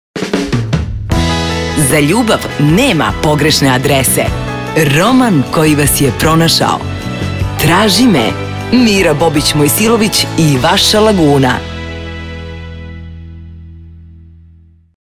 Trejler za knjigu „Traži me“, Mirjane bobić Mojsilović
Ko traži, naći će...

Jedan vetropir, jedna obična žena, jedna slavna književnica – junaci su ove priče koja prati tri naizgled potpuno različite sudbine, koje, međutim, imaju nešto zajedničko. Svi oni prolaze kroz život bez istinskog suočavanja sa pitanjima ko sam, šta želim, kuda idem i da li moj život liči na onaj o kojem sam sanjao.

Da li uspomene mirišu, da li je uspeh kao ksanaks, da li je Ada beogradski Varadero, i šta je to što sve troje junaka traži pročitaćete u ovom modernom i veoma uzbudljivom romanu, u kojem svako može da prepozna i deo svoje sudbine.